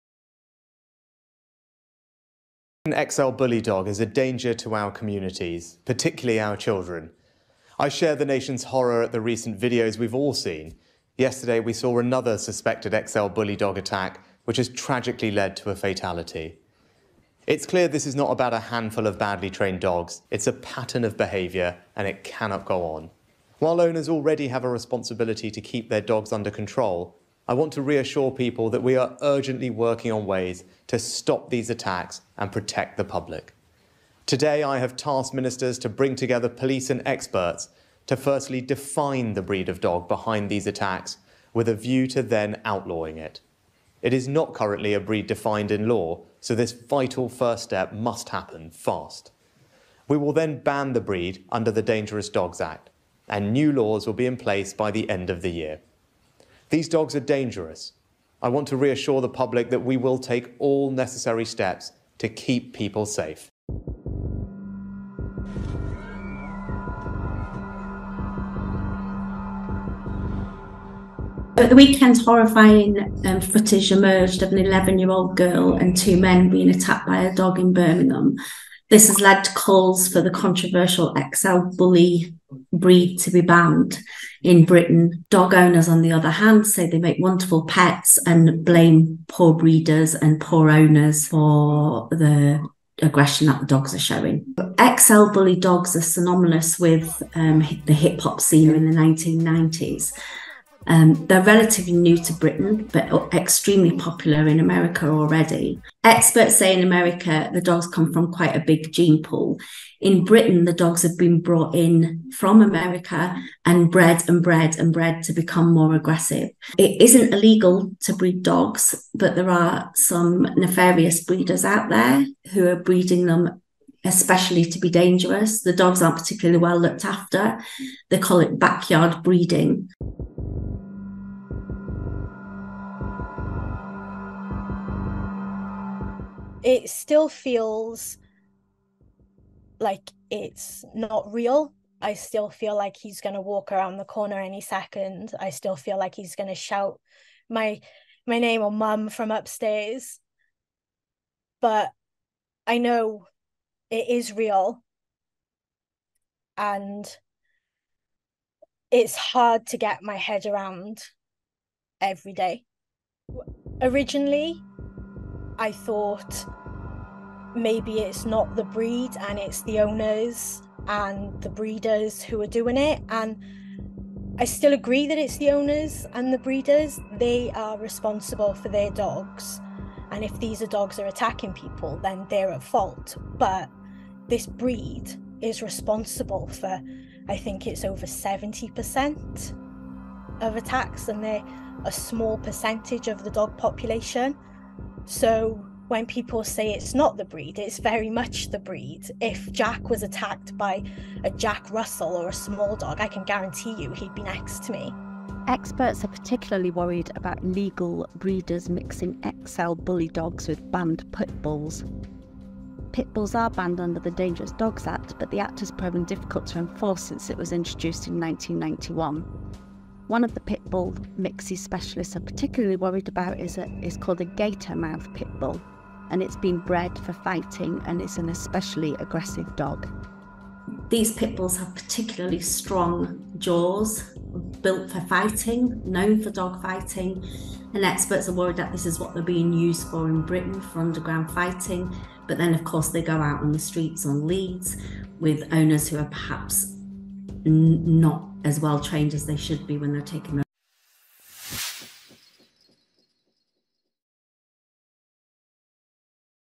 breaking news and Politics RISHI SUNAK BANS XL BULLY DOGS AFTER INCIDENT
JOIN THE CONVERSATION & PUT YOUR COMMENTS BELOW